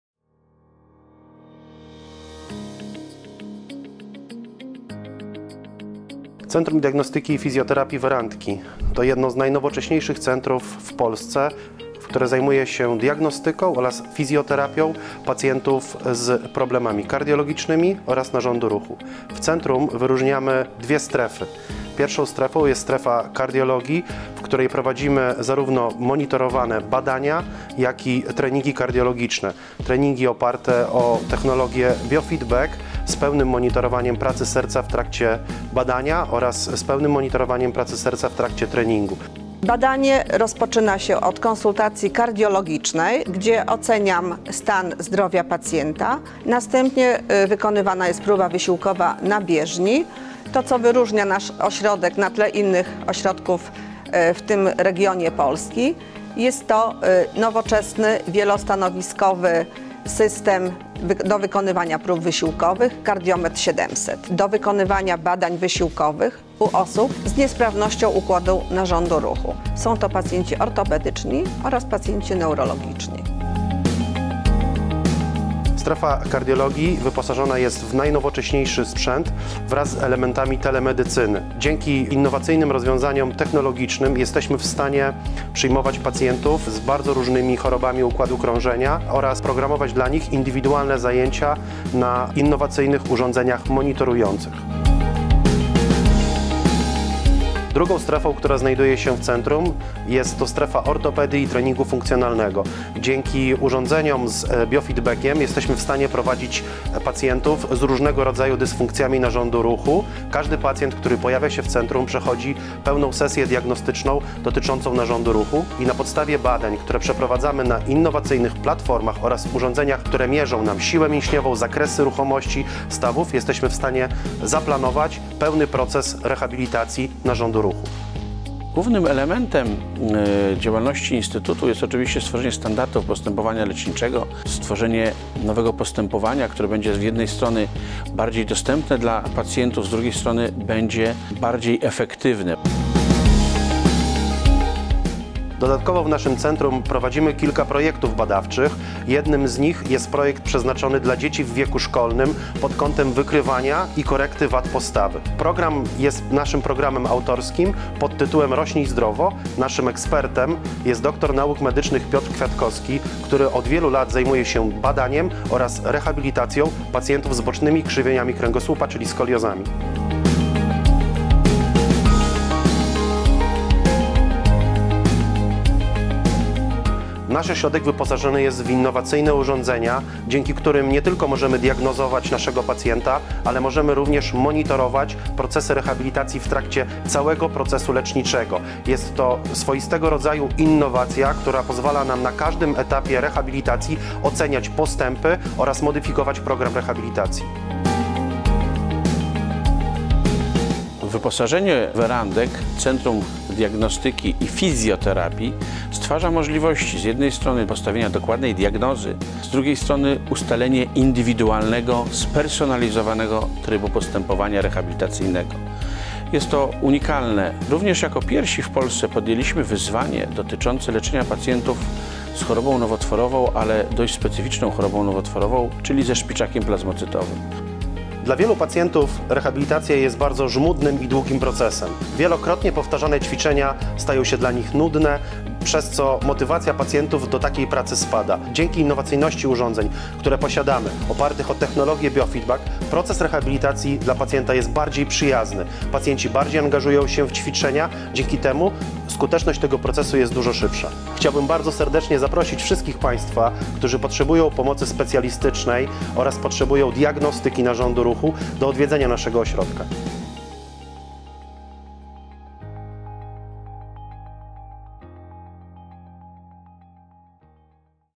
Werandki - Uzdrowisko Nałęczów. Nowoczesne centrum diagnostyki i rehabilitacji
Centrum Diagnostyki i Fizjoterapii „Werandki” zostanie oficjalnie otwarte dla kuracjuszy już 24 maja 2018 roku , i będzie kompleksowo obsługiwało cztery obszary rehabilitacji medycznej: Strefę Kardiologii, Strefę Ortopedii, Strefę Treningu oraz Strefę Diagnostyki.

Cały sprzęt do Uzdrowiska Nałęczów - Werandki dostarczył Technomex.